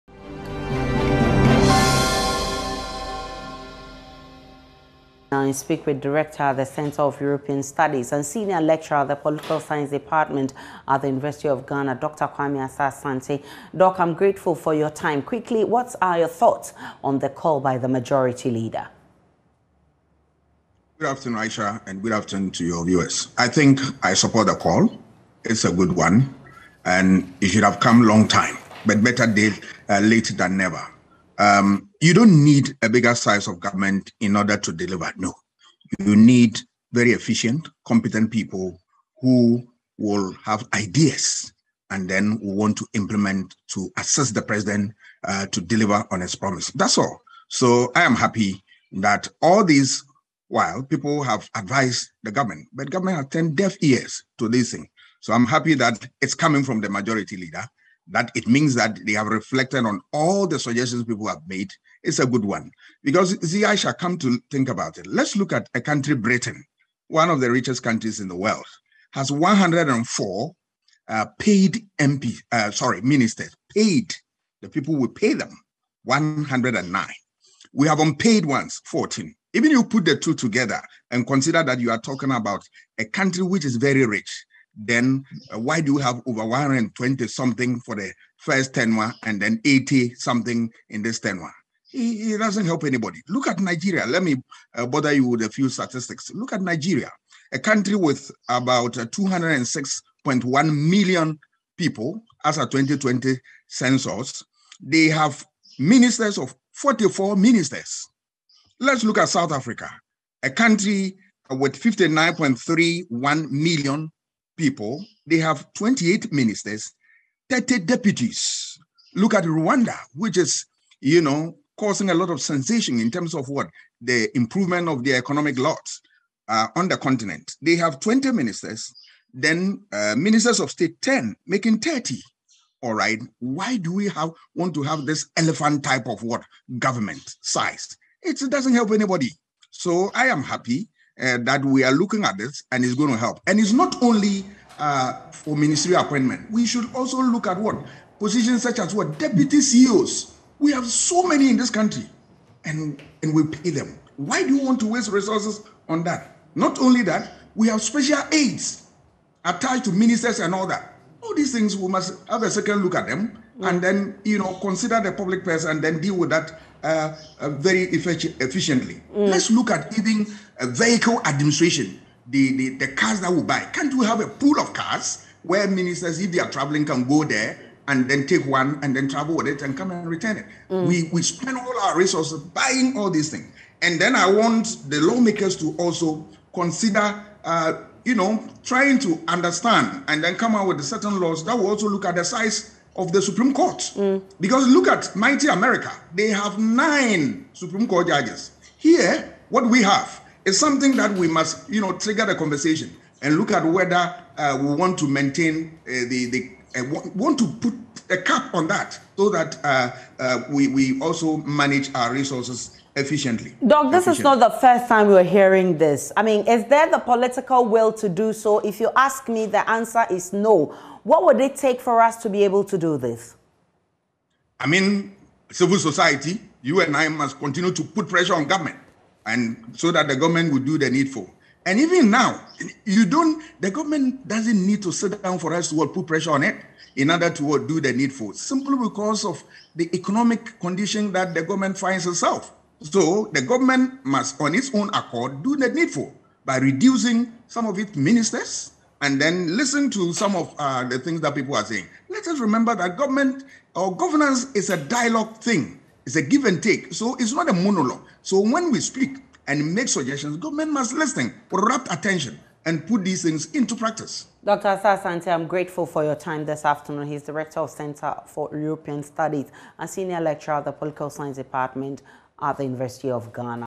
Keeping a Lean Government: We need efficient ministries, not a large number – Dr. Asah-Asante
We need efficient ministries, not a large number – Dr. Asah-Asante - Joy News Today